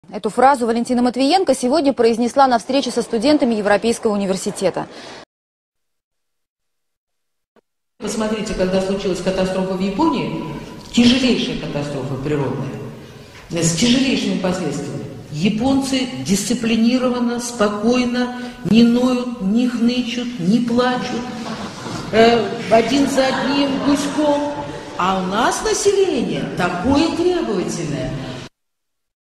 Жесть Матвиенко: "А у нас население такое требовательное!" А жертвам Крымска сказала бы это в лицо?
Жесть. Валентина Матвиенко, губернатор Санкт-Петербурга, а ныне Председатель Совета Федерации, выступает 19 мая 2011 года.

Валентина Матвиенко: "...А у нас население такое требовательное!..."

А жертвам Крымска сказала бы Валентина Матвиенко эти слова в лицо?!

Жертвам наводнения в Крымске лучше не смотреть и не показывать!

Встреча со студентами Европейского университета в Санкт-Петербурге 19 мая 2011 года - реакция Валентины Матвиенко на вопрос о положении в Санкт-Петербурге предыдущей зимой, когда прошли обильные снегопады.

Валентина Матвиенко: «Посмотрите, когда случилась катастрофа в Японии, тяжелейшая катастрофа природная, с тяжелейшими последствиями, японцы дисциплинированно, спокойно, не ноют, не хнычут, не плачут, один за одним, гуськом... — а у нас население такое требовательное!» © Матвиенко

Валентина Матвиенко  выражает недовольство тем, как ведут себя в России жертвы природных или техногенных катастроф в момент аварий и стихийных бедствий. Не так себя ведут жертвы катастроф. Чиновница Валентина Матвиенко открыто недовольна попавшими в передряги избирателями Российской Федерации. редкое видео rare video

Весь сюжет целиком можно посмотреть на этом же канале

Матвиенко. Жертвам Крымска не показывать! Matvienko, Russia

Эта цитата из слов Валентины Матвиенко в репортаже новостей по телевидению взята полностью от первого до последнего звука и жеста. Я бы, может быть, и хотел больше показать Валентину Матвиенко, продолжение фразы, что она говорила, так не из чего! В репортаже по телевидению продолжения её выступления не было и нет. В словах Матвиенко нет никакой лично моей нарезки.

Кроме того, не случайно, я думаю, вся видеоинформация про это  выступление Валентины Матвиенко везде в Интернете уничтожается.

          Valentina Matvienko, Governor of Saint-Petersburg,  now Chairman of the Federation Council of Russia does not like how people in Russia react in the case of emergency. rare video редкое видео

matvienko_about_victims_russians_compare_with_japanese_talk_students_spb_euro_university6.wmv